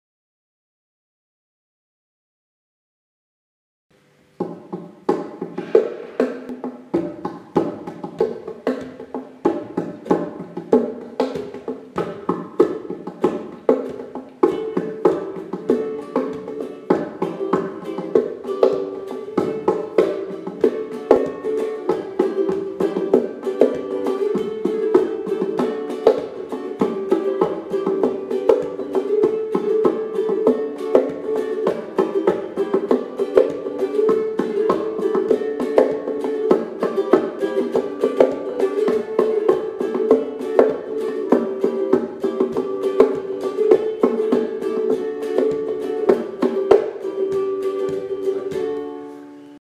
Fluper
Fluper is the result of a design concept which was generated after studying percussion and wind instruments. It is a creative intervention in which elements of the drums and the flute were merged.This solo project  is made out of PVC pipes.